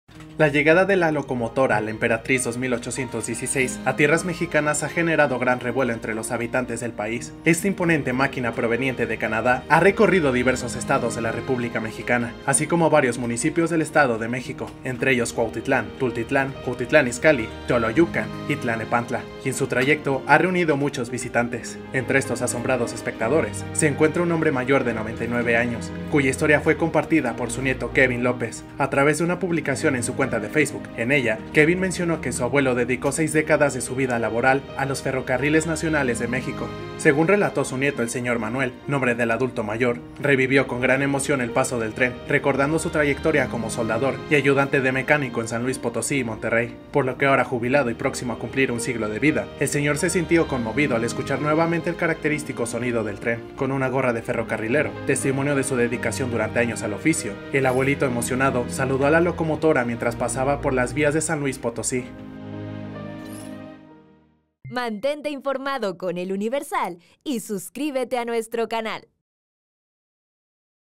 Exmaquinista de 99 años se despide de “La Emperatriz” en San Luis Potosí
Un exmaquinista de 99 años, que dedicó 60 años de su vida laboral a los Ferrocarriles Nacionales de México, se despidió con emoción de la locomotora “La Emperatriz” en San Luis Potosí